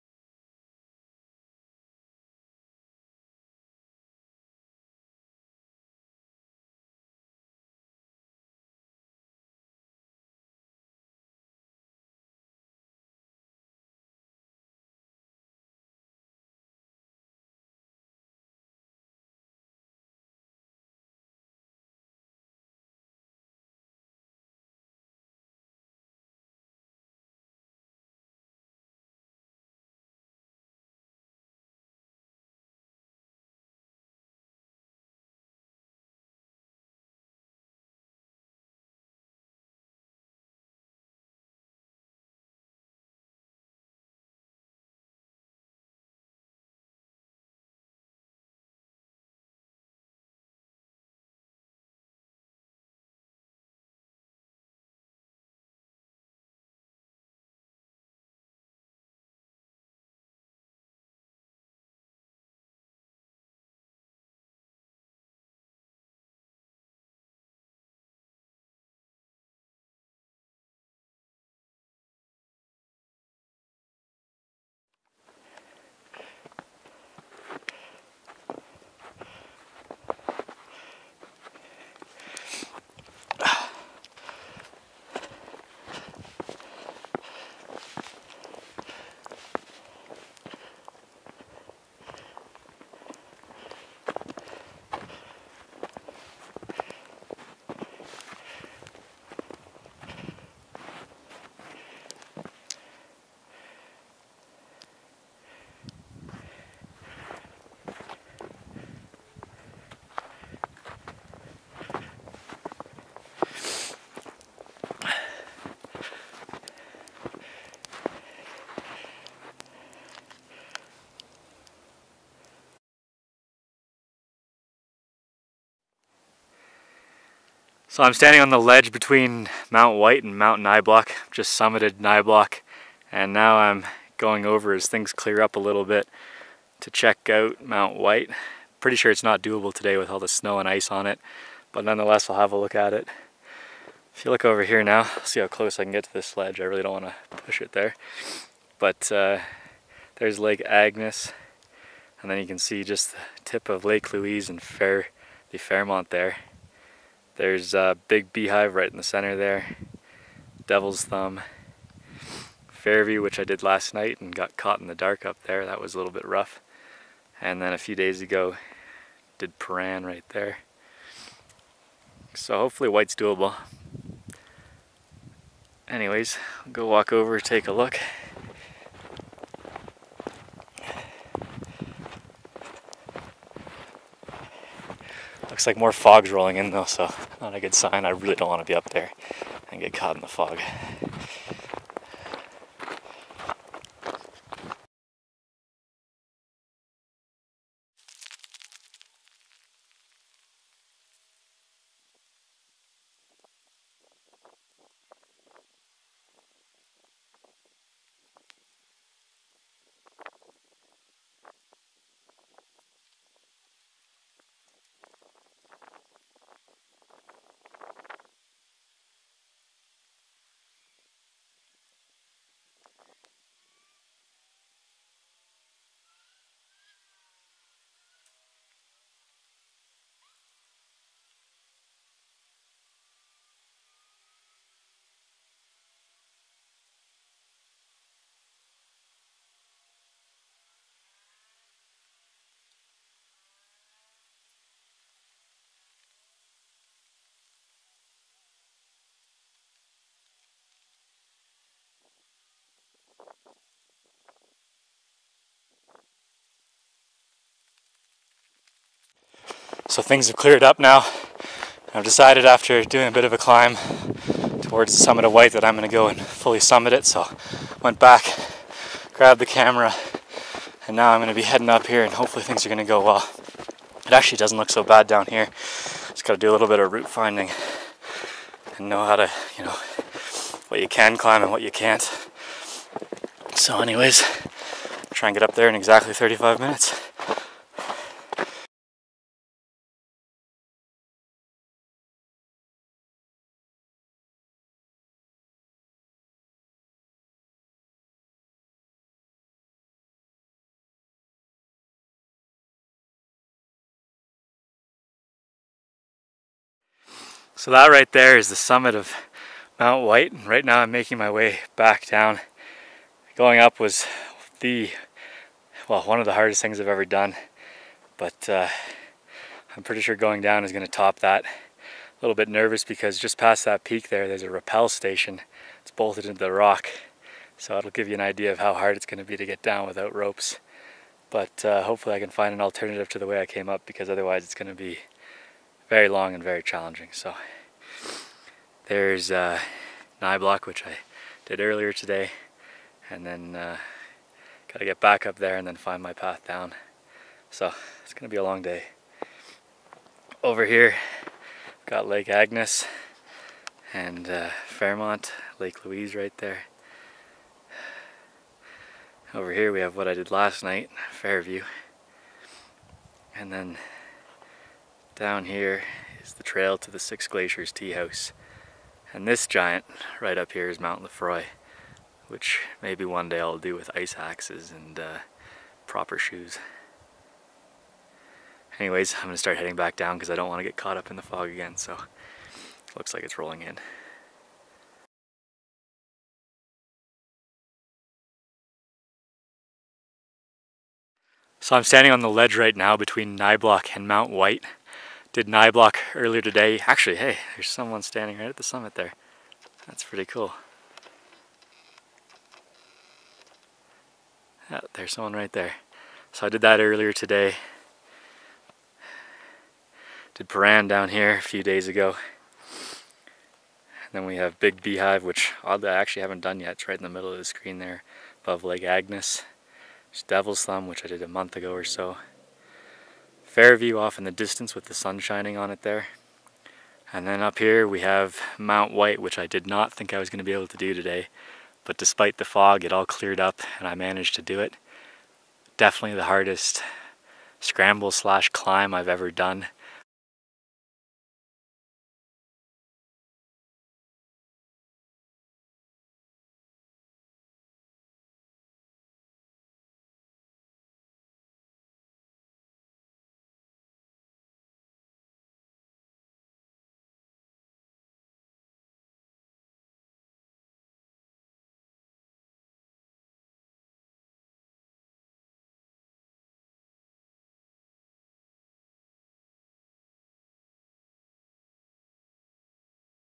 Mt. Niblock and Mt. Whyte
Scrambled up to Mt. Niblock & Mt. Whyte on a cloudy day. September 4, 2016.

Round trip time was around 9 hours.